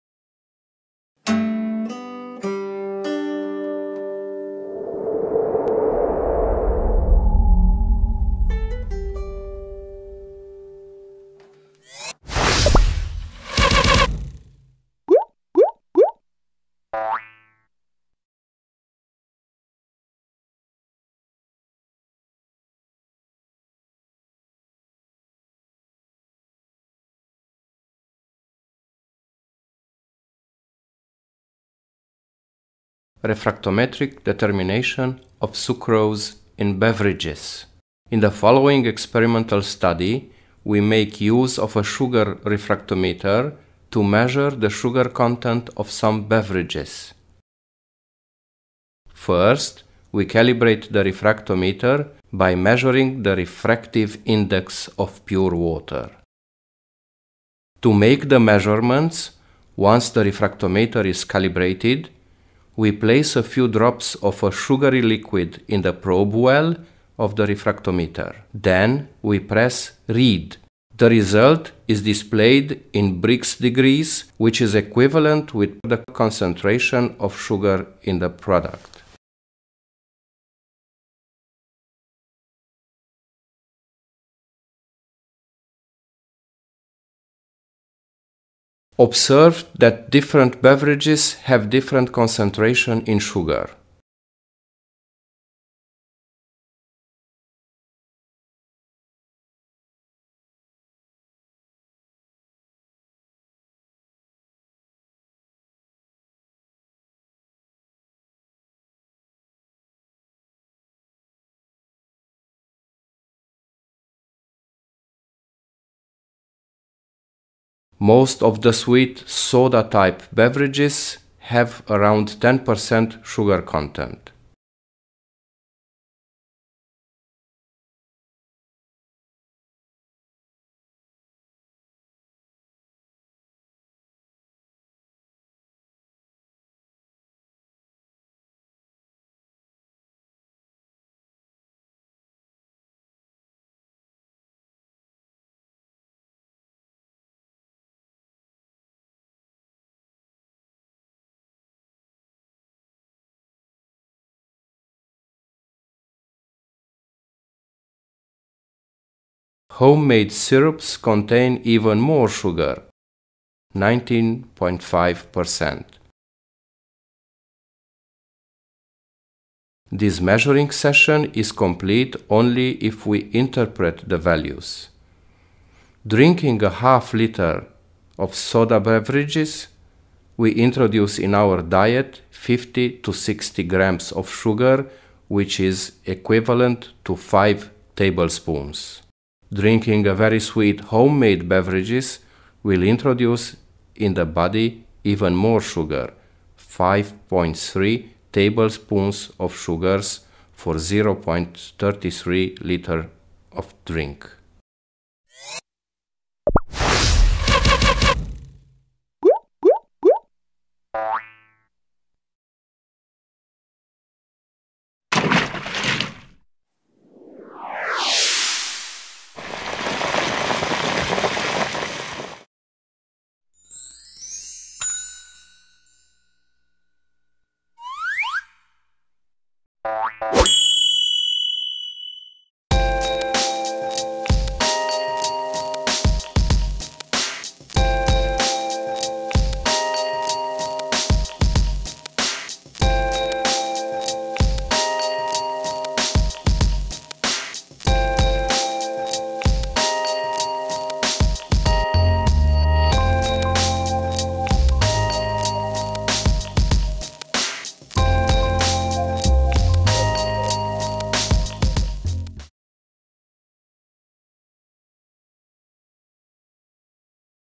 REFRACTOMETRIC DETERMINATION OF SUGARS IN BEVERAGES
A specially calibrated refractometer helps us to determine the sugar content of different beverages. A further analysis reveals the huge amounts of sugar intorduced in the body with some beverages.